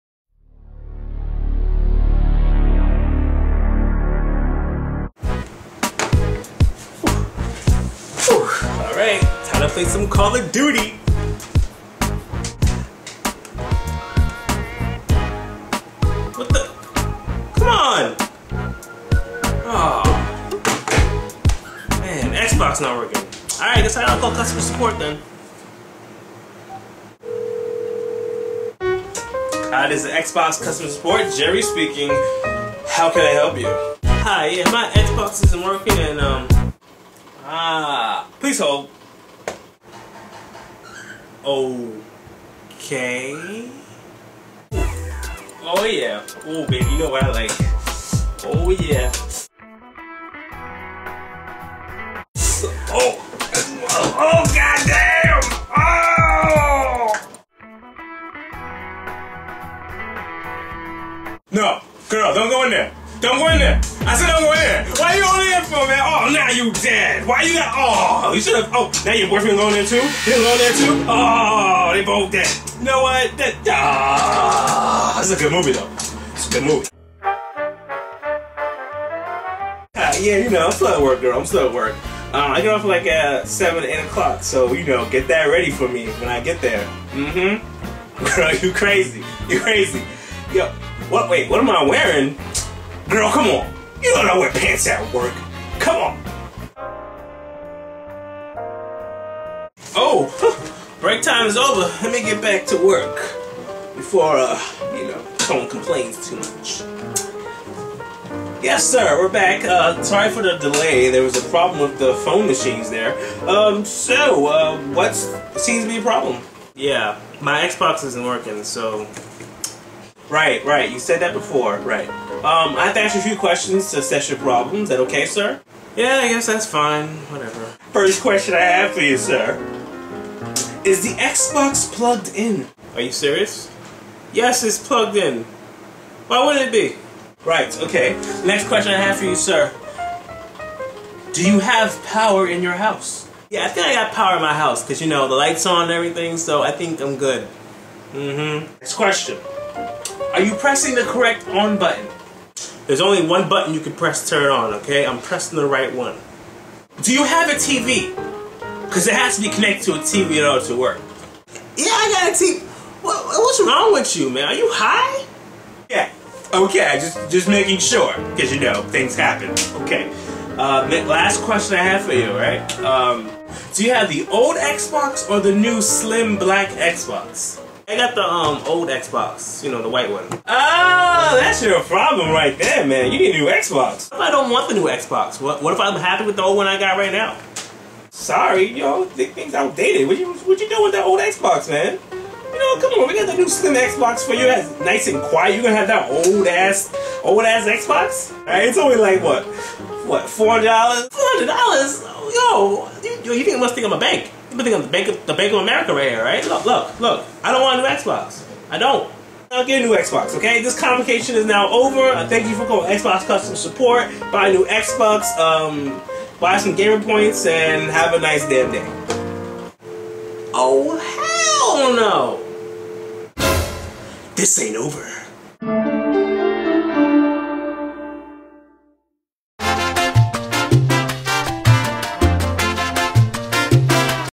What phone operators REALLY DO when they put you on hold!
Ever had to deal with customer service over the phone? Of course you have, this video finally exposes their real activities while you're waiting on hold listening to that boring elevator music...enjoy!